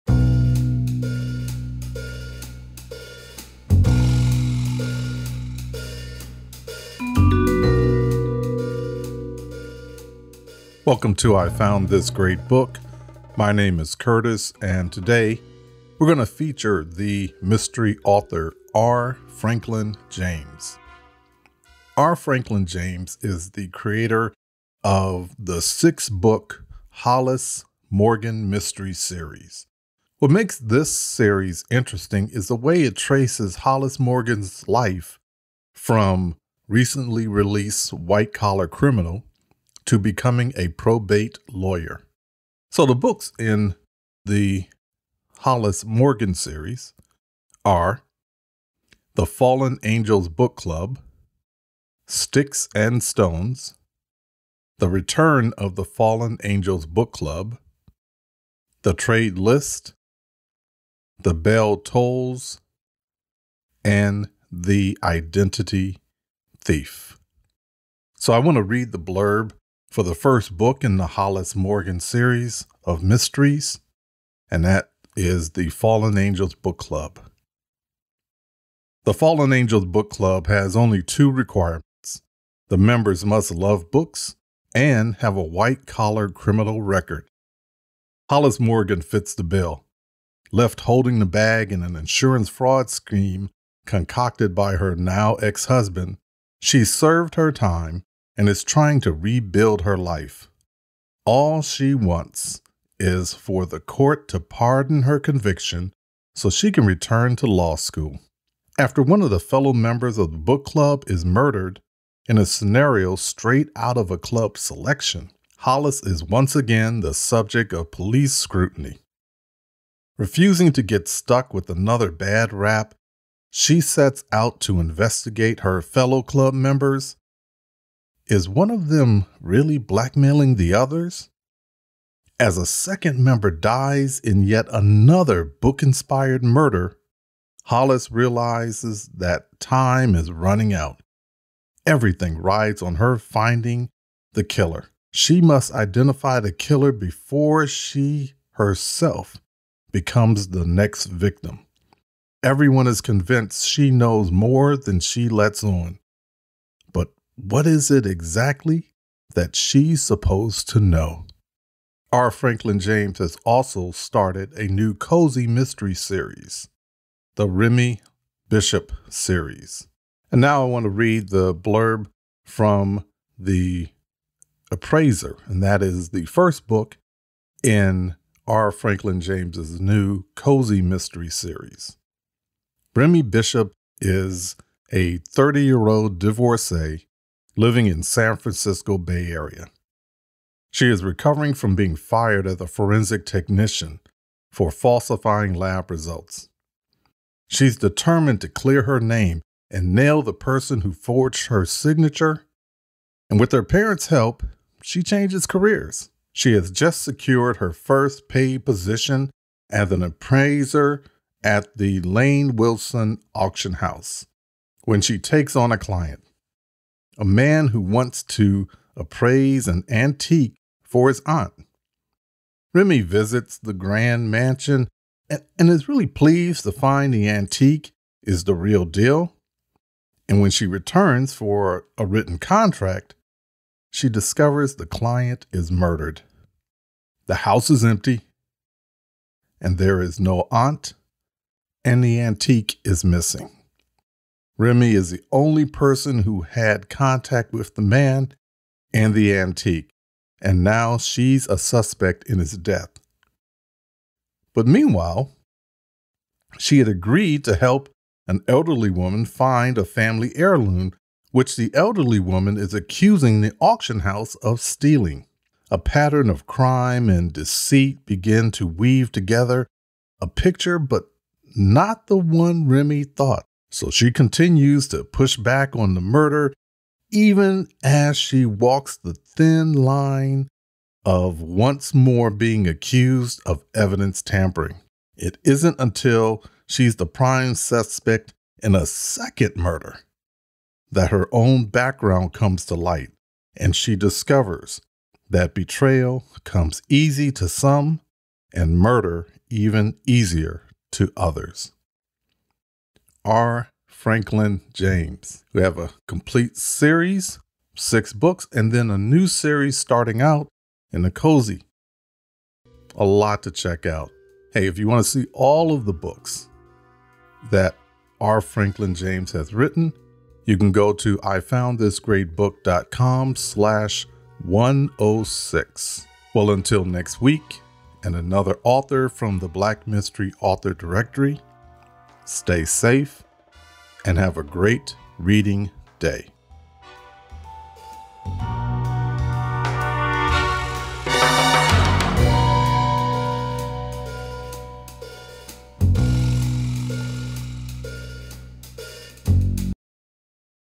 R. Franklin James - Featured Mystery Author
This week's featured author is R. Franklin James. The creator of the Hollis Morgan mystery series. A six book series that follows the life of Hollis Morgan from recently released white collar criminal to becoming a probate lawyer.

 The books in this series are:

 The Fallen Angels Book Club

 Sticks &amp; Stones

 The Return of the Fallen Angels Book Club

 The Trade List

 The Bell Tolls

 The Identity Thief

 R. Franklin James has started a new cozy mystery series. The first book in the series is: The Appraiser.